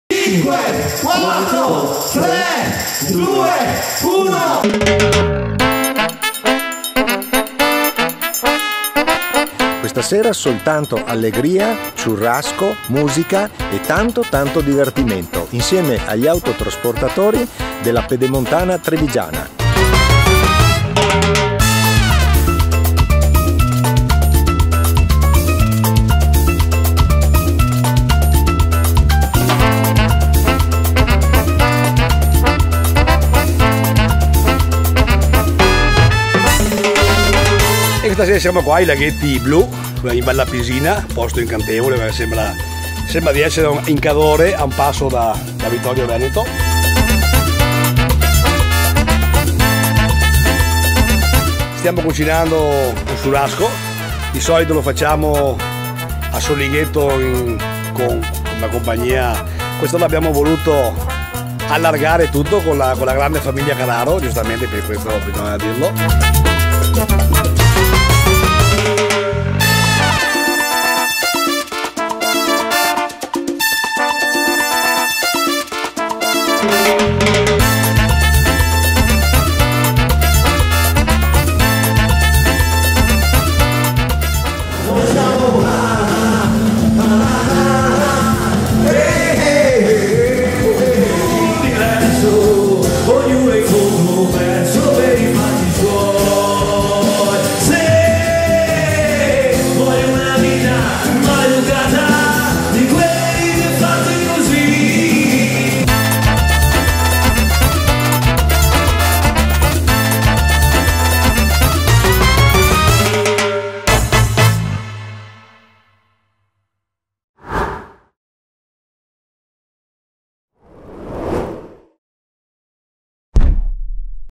Serata del Churrasco
Sabato 15 luglio 2017 lo staff dei trucks di Carraro si è unito al Churrasco di Fabio Decet del gruppo autotrasportatori della pedemontana trevigiana. Per una volta, niente test drive, nessuna prova di pieno carico, solo allegria e spensieratezza, con il sound cabarock di Frank e Beppe.